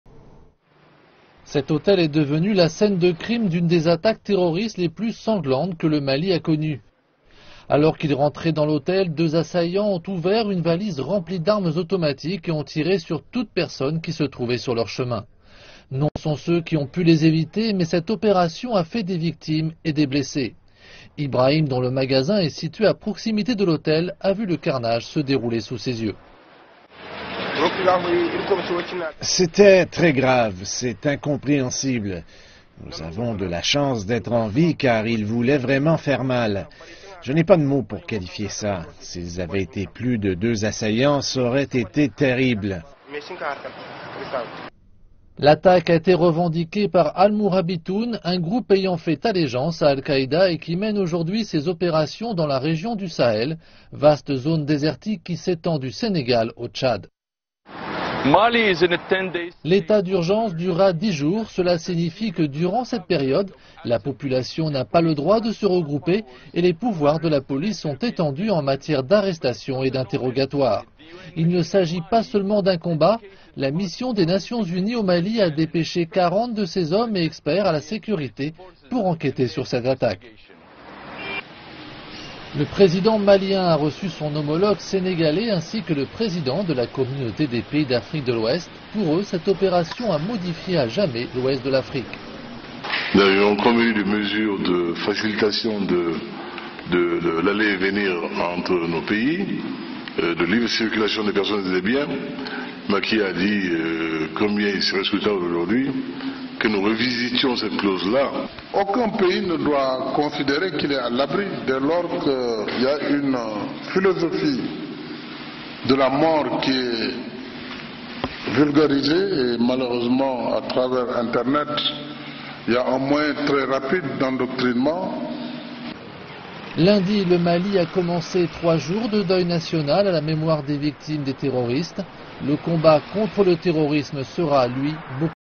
Deuil national et état d’urgence：l’Afrique de l’Ouest sous le choc
Au Mali, les 3 jours de deuil national ont officiellement débuté lundi. Le pays rend hommage aux 21 personnes tuées durant l'attaque de l'hôtel Radisson Blue. Au Mali et dans d'autres pays d'Afrique de l'Ouest, les autorités sont en état d'alerte. L'opération terroriste devrait avoir de larges conséquences sur les liens unissant les pays de la région.